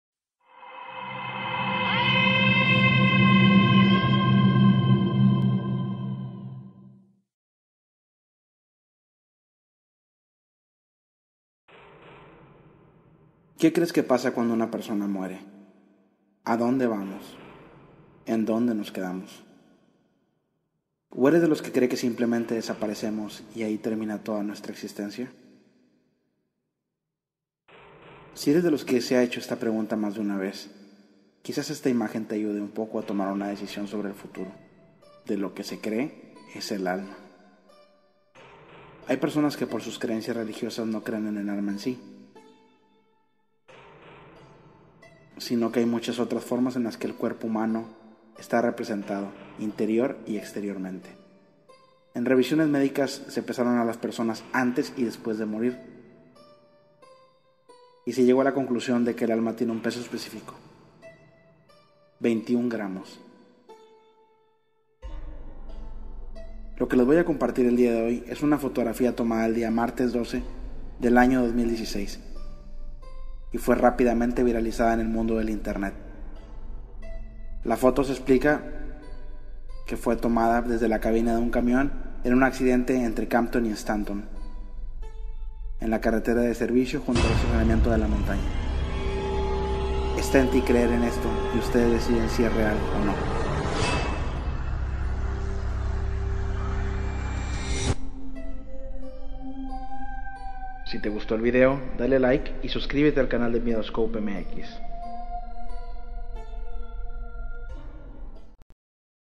A dónde va el alma? Foto de un Alma abandonando el cuerpo VIRAL
Les comparto una foto y con ella una historia que se esta viralizando en estos días sobre un alma abandonando un cuerpo y esto fue captado en una fotografía.